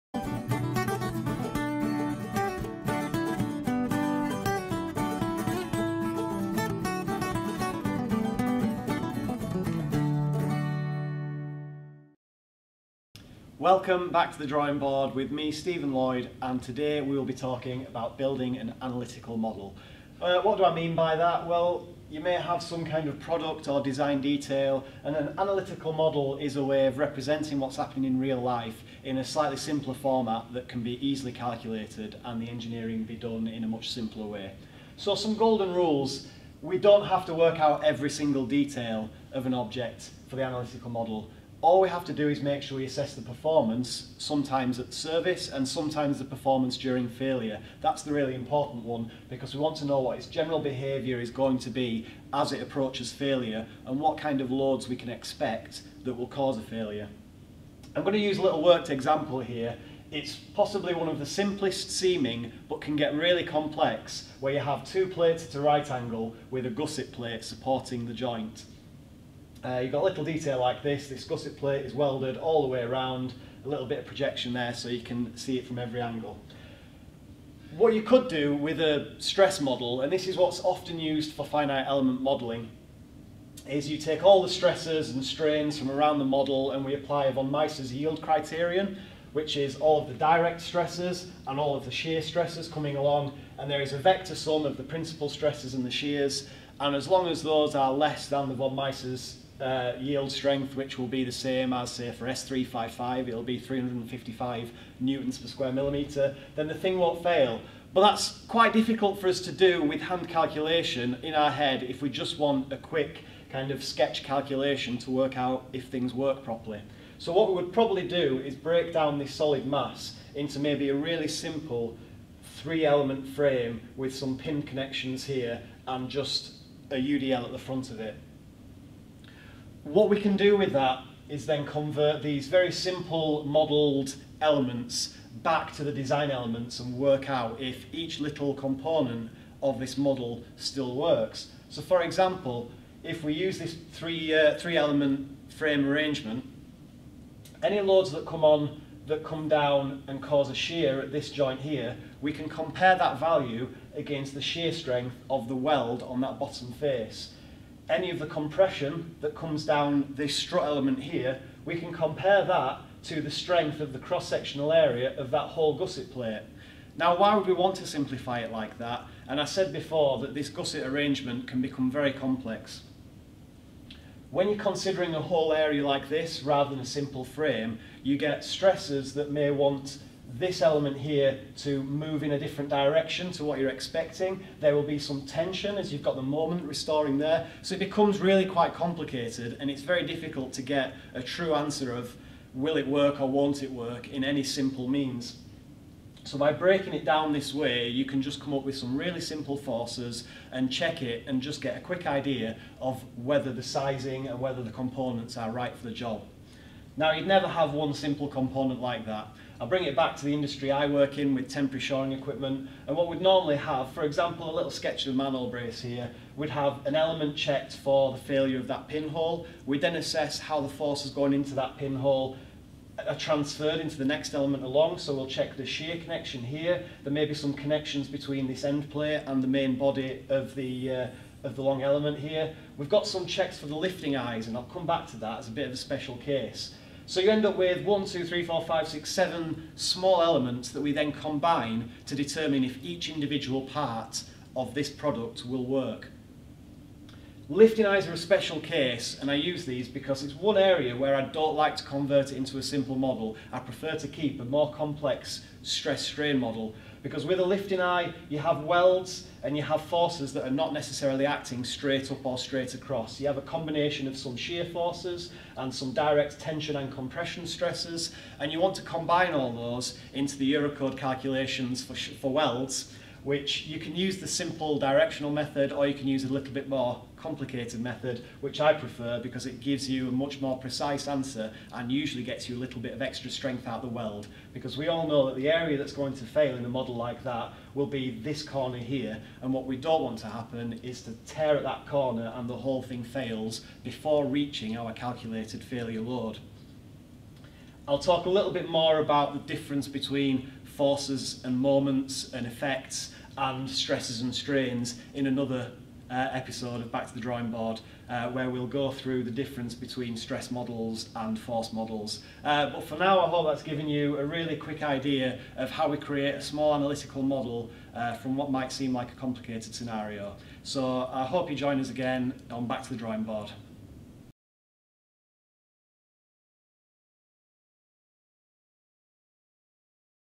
Building an Analytical Model | Back to the Drawing Board
Welcome back to Back to the Drawing Board, where Steven Lloyd joins us to talk through the topic of Building an Analytical Model.

Let us know your thoughts on this topic or any suggestions for another video below.